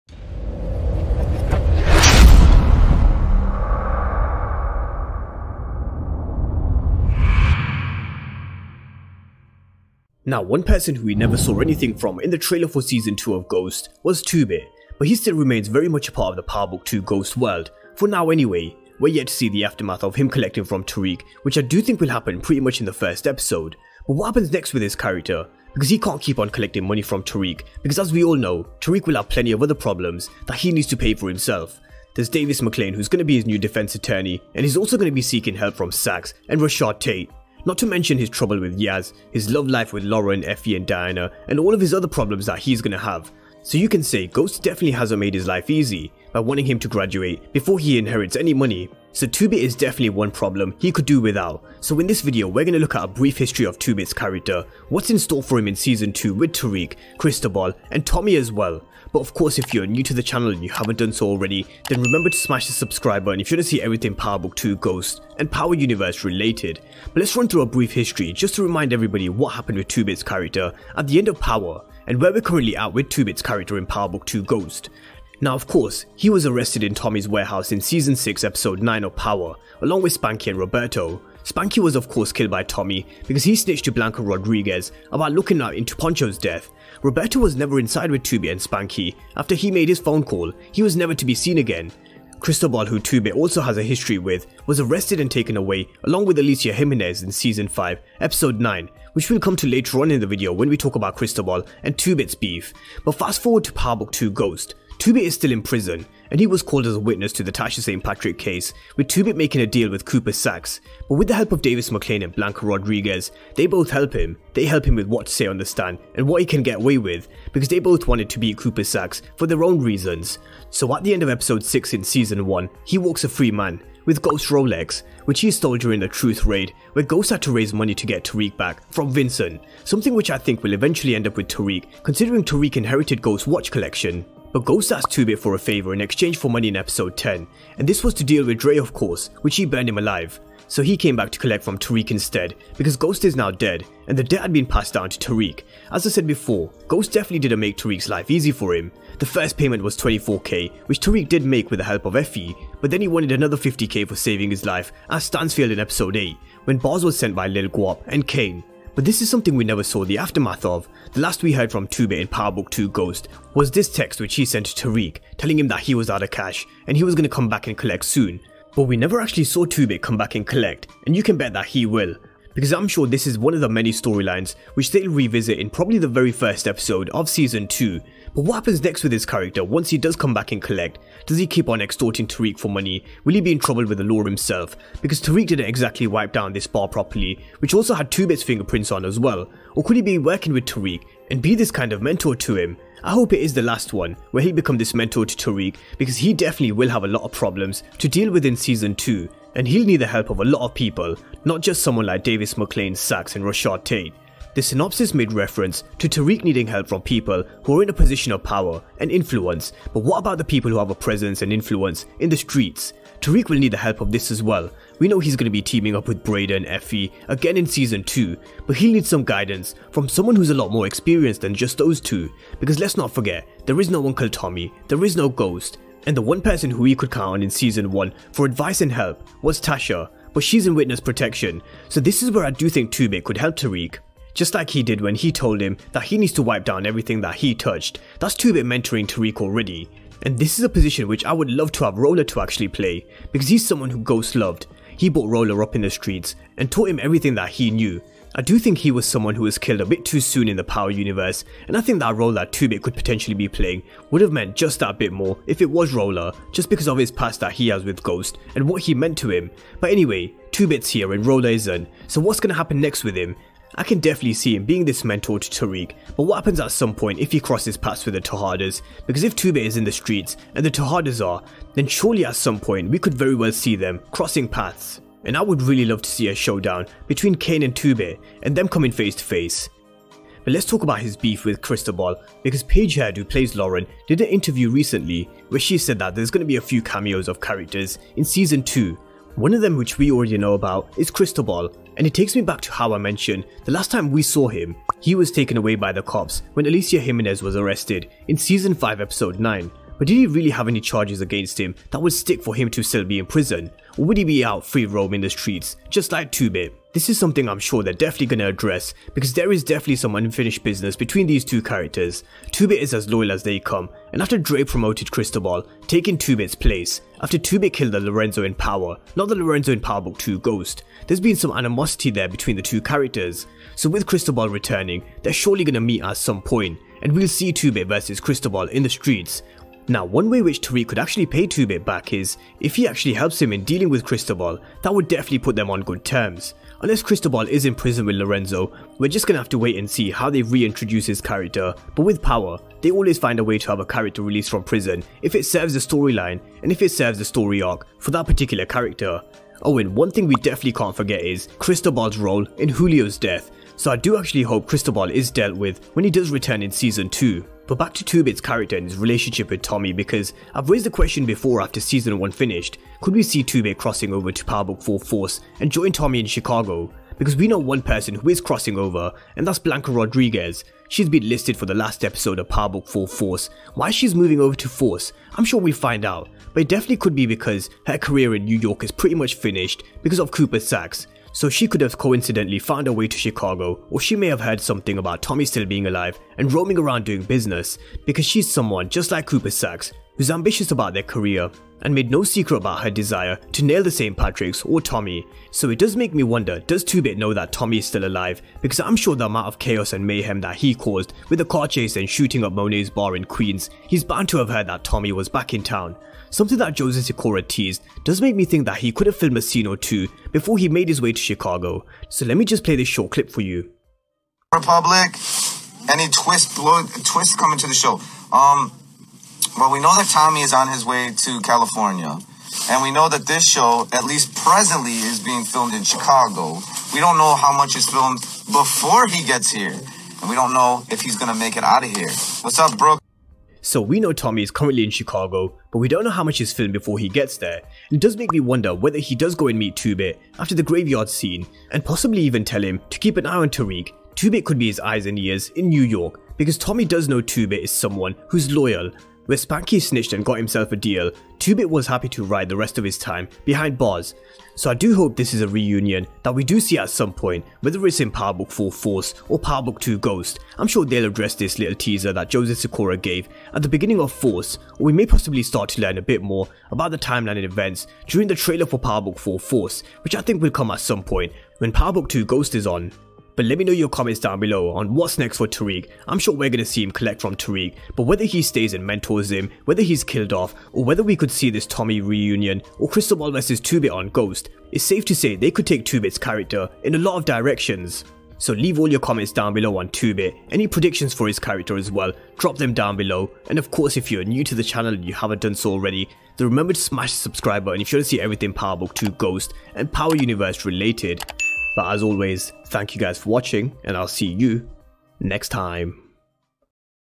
2-Bit WILL BE BACK To Collect But What’s Next? | Power Book 2 Ghost Season 2 Theories
During episode 6 of Power Book 2 Ghost Season 1, we saw the return of 2-Bit.  He was helped by Blanca Rodriguez and Davis Maclean and was released from prison.  He came back to collect from Tariq, a debt which Ghost owed him, which has now been passed down to Tariq.  We never saw the aftermath of 2-Bit helping Tariq at Stansfield with Barz, but you can be sure that he'll be back to collect from Tariq, because he's out of cash and he told Tariq that he was going to be back soon.  But what's next for his character when he does return and collect from 2-Bit?  I discuss a few possibilities, because for someone like 2-Bit, the possibilities are endless for his character with what they could do.